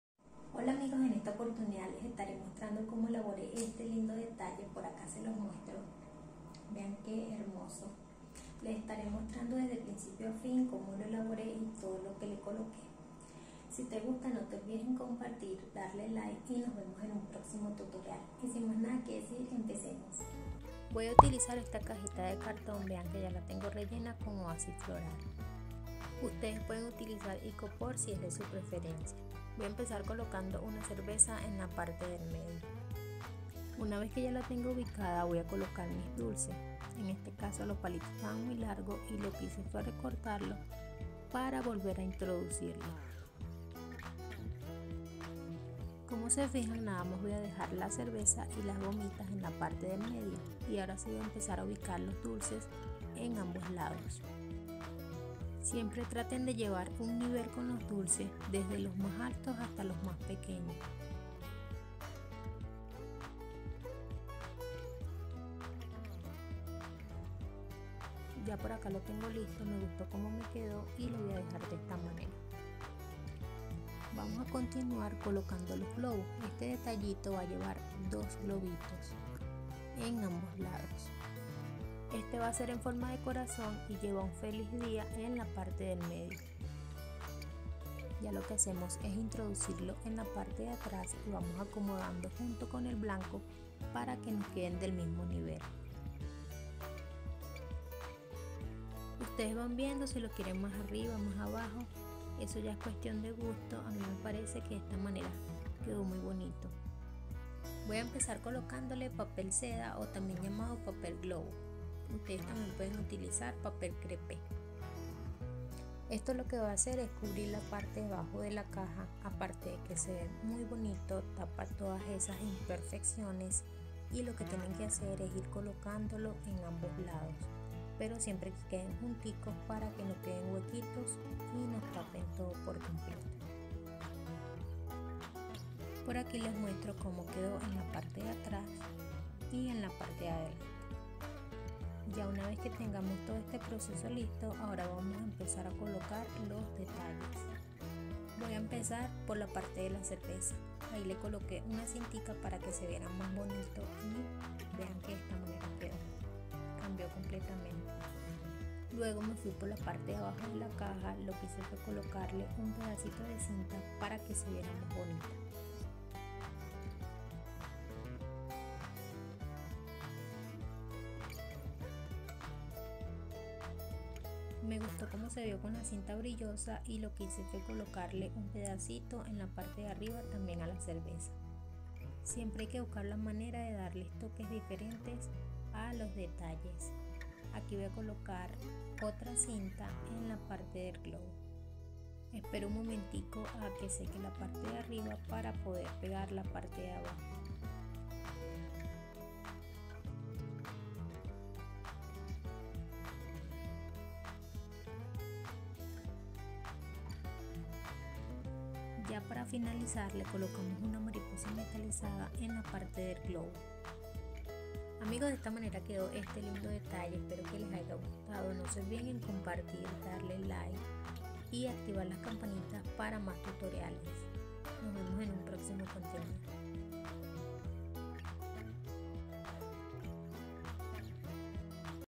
Cómo hacer un lindo detalle, te enseño el paso a paso para que puedas elaborar tus regalos💝🎁🎀🍻
Te muestro y te explico paso a paso como elabore este lindo detalle, acompáñame para que puedas aprender y obseguiar a tus seres queridos detalles hecho por ti 😍🎀💝🎁